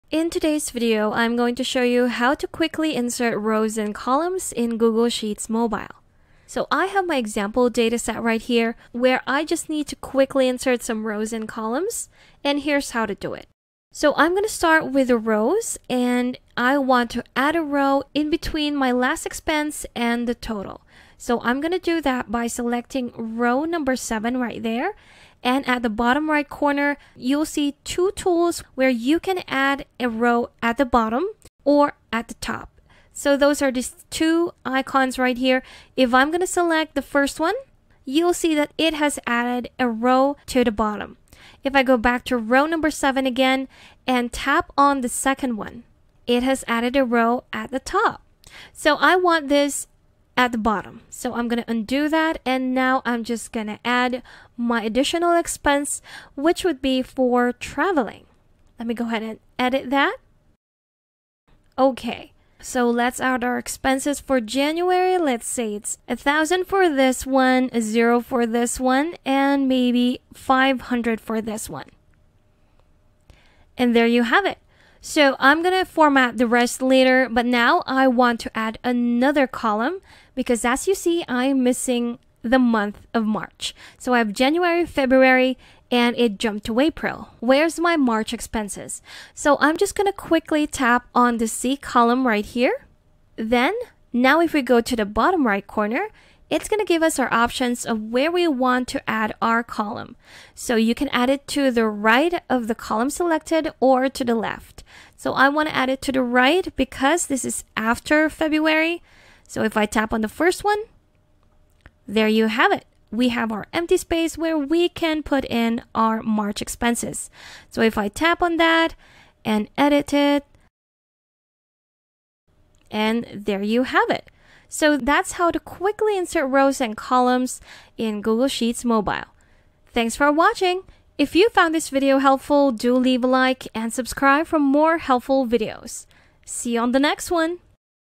Quickly Insert Rows and Columns in Google Sheets Mobile (Quick Video)
Learn how to quickly insert rows and columns in Google Sheets on your mobile device!

In this video, I’ll show you how to efficiently add rows and columns with just a few taps, perfect for managing data on the go or making quick updates. Follow this step-by-step guide to streamline your workflow and become a Google Sheets mobile pro in no time. Ideal for students, professionals, and anyone needing to use Google Sheets on their smartphone or tablet.